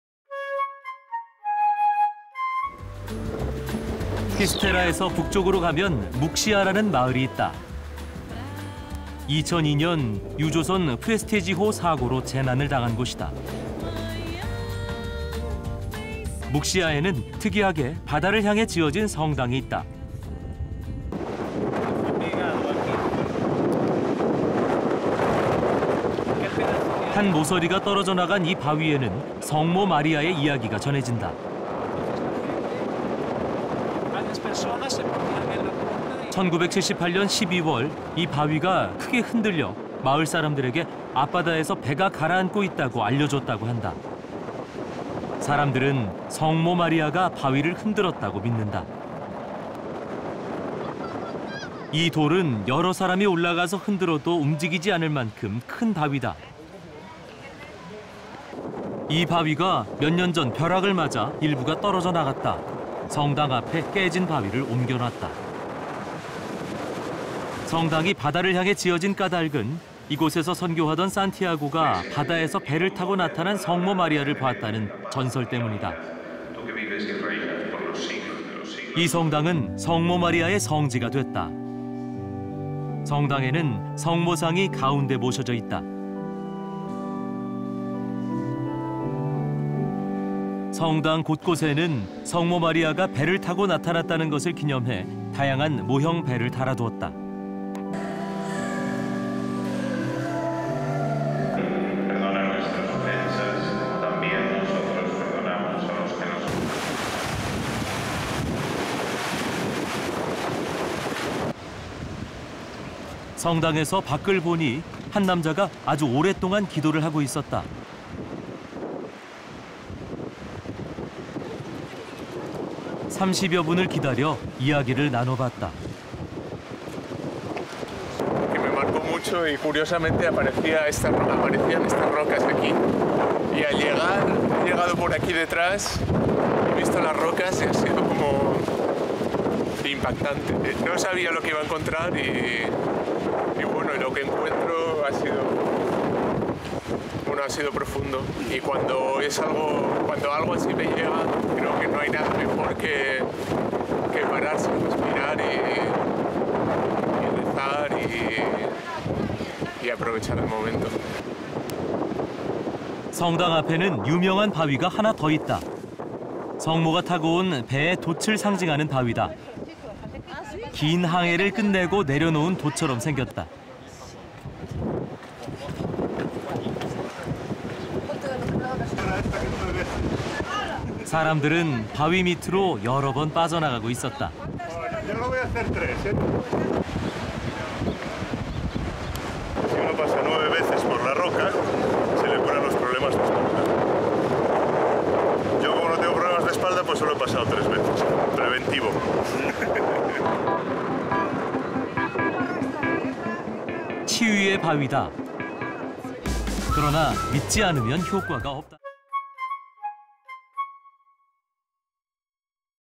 【K】Spain Travel-Muxia[스페인 여행-묵시아]성모마리아의 성지 묵시아/Mary/Shrine/Catholic church/Nosa Senhora da Barca
[한국어 정보]
피스테라에서 북쪽으로 가면 묵시아라는 마을이 있다. 2002년 유조선 프레스티지호 사고로 재난을 당한 곳이다. 묵시아에는 특이하게 바다를 향해 지어진 성당이 있다. 한 모서리가 떨어져 나간 이 바위에는 성모 마리아의 이야기가 전해진다. 1978년 12월 이 바위가 크게 흔들려 마을 사람들에게 앞바다에서 배가 가라앉고 있다고 알려주었다고 한다. 사람들은 성모 마리아가 바위를 흔들었다고 믿는다. 이 돌은 여러 사람이 올라가서 흔들어도 움직이지 않을 만큼 큰 바위다. 이 바위가 몇 년 전 벼락을 맞아 일부가 떨어져 나갔다. 성당 앞에 깨진 바위를 옮겨 놓았다. 성당이 바다를 향해 지어진 까닭은 이곳에서 선교하던 산티아고가 바다에서 배를 타고 나타난 성모마리아를 보았다는 전설 때문이다. 이 성당은 성모마리아의 성지가 됐다. 성당에는 성모상이 가운데 모셔져있다. 성당 곳곳에는 성모마리아가 배를 타고 나타났다는 것을 기념해 다양한 모형 배를 달아두었다. 성당에서 밖을 보니 한 남자가 아주 오랫동안 기도를 하고 있었다. 30여분을 기다려 이야기를 나눠봤다. “몇 주 전에 꿈을 꾸었는데요. 신기하게도 꿈에 저 바위들이 나왔어요. 제가 여기 뒤쪽으로 도착했는데 저 바위들을 본 순간 충격이었지요. 이곳에 와서 무얼 보게 될 줄 몰랐는데 정말 엄청난 것을 발견하고 말았죠. 이런 순간이 찾아올 때 가장 좋은 것은 멈춰 서서 숨을 들이쉬고 기도하고 그 순간을 즐기는 것이라 생각해요.” 성당 앞에는 유명한 바위가 하나 더 있다. 성모가 타고 온 배의 돛을 상징하는 바위다. 긴 항해를 끝내고 내려놓은 돛처럼 생겼다. 사람들은 바위 밑으로 여러 번 빠져나가고 있었다. “저 바위 밑을 9번 지나면 허리 문제가 해결됩니다. 저는 허리가 안 아파서 3번만 했어요. 예방차원에서요.” 치유의 바위다. 그러나 믿지 않으면 효과가 없단다.

Go in peace tailored to the north is the village of Ara Apocalypse. 2002 is where the tanker Prestige suffered a disaster to call the accident. Oh, there are implied unusual cathedral was built towards the sea. This is a corner away from rock passed out the story of the Virgin Mary. In December 1978 it gave us the ship that sank in waters off the rocks by villagers greatly shaken. Eotdago people believe that the Virgin Mary to shake the rocks. These stones are large enough to move rocks several people climbed shaking. The rock broke off the right part of the lightning a few years ago. Replace the broken rocks placed in front of the cathedral. The cathedral was built towards the sea therefore saw the Virgin Mary appeared here in Santiago was the mission aboard the ship in the sea, because the legend. The Cathedral of the Virgin Mary was a holy place. The cathedral has a statue enshrined center. The cathedral was placed around the various models boat weighed in commemoration of the Virgin Mary that appeared aboard. A man looked out at the cathedral were praying for a very long time. Wait for 30 minutes looked share stories. ""Neundeyo had a dream a few weeks ago. Curiously got that rock their dreams. I had arrived back here this instant eotjiyo the impact of that rock. I did not come here to see'd be doing really great find and malatjyo. I think this moment would come when the stand is the best stop praying breath and enjoy the moment. ""In front of the famous Cathedral Rock is one more. The ship sails on a rock to symbolize the Virgin rides. End the long voyage down the place looks like a sail. People were passing through multiple rock bottom. ""After solving the problem will be back under that rock 9. I got back is not only sick three. Y'know precaution. ""The rock of healing. If you do not believe, however, eopdanda effect.

[Information]
■클립명: 유럽099-스페인15-16 성모마리아의 성지 묵시아/Mary/Shrine/Catholic church/Nosa Senhora da Barca
■여행, 촬영, 편집, 원고: 성수일 PD (travel, filming, editing, writing: KBS TV Producer)